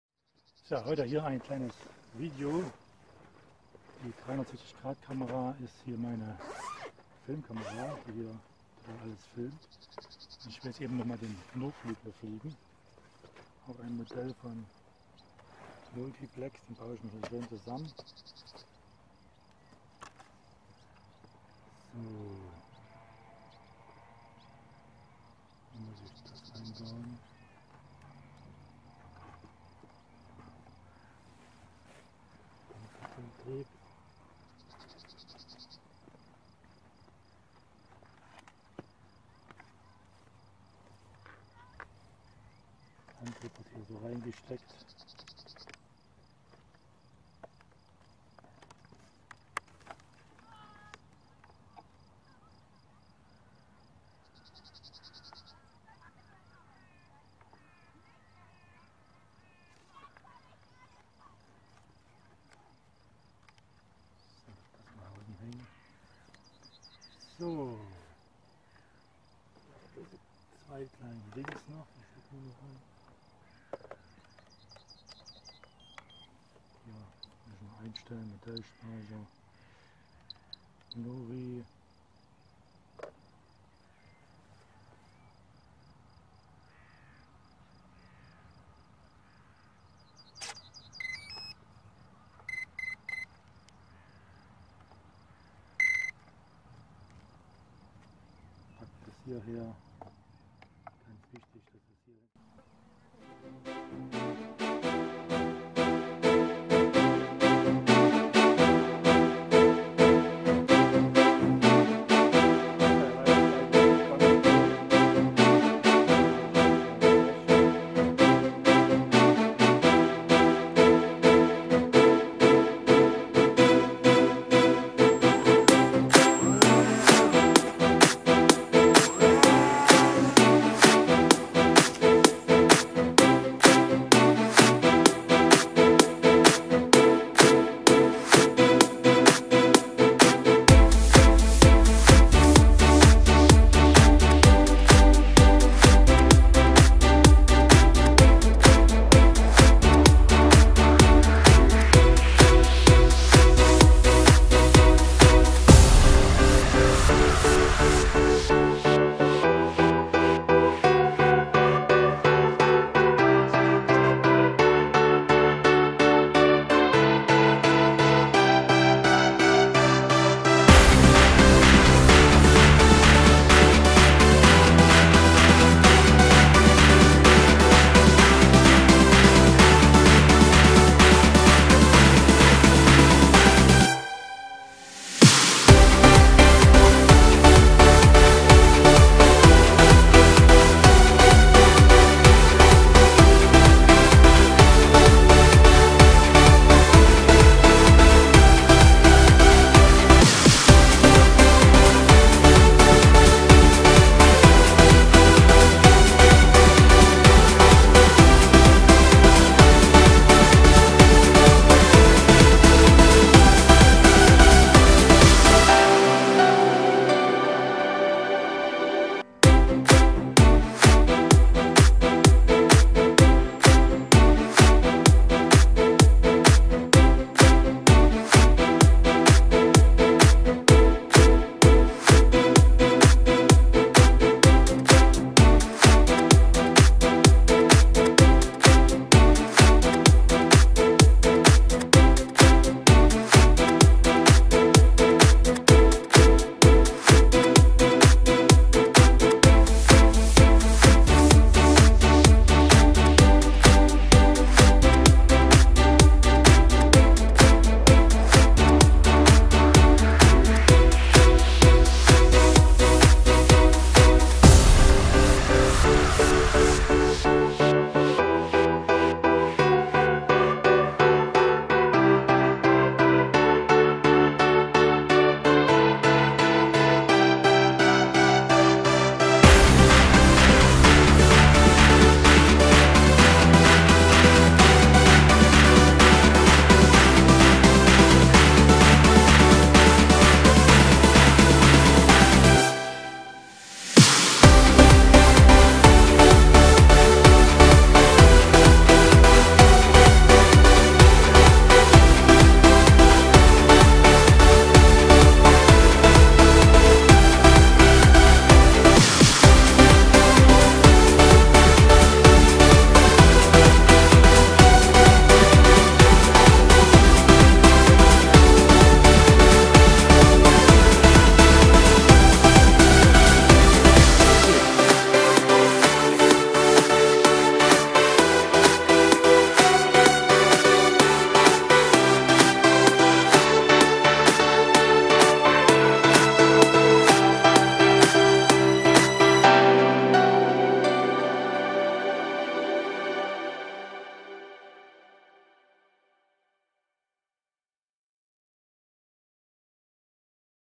Samsung Gear 360: 4 K MPX Xeno with Mobius C Lens Onbord 1080i
Hier im Video habe ich die 360° Film mit den Onboardfilm 1080! des Nurflüglers kombiniert versehen.
Here in the video I have the 360 ° film with the onboard movie 1080! the flying wing combination provided.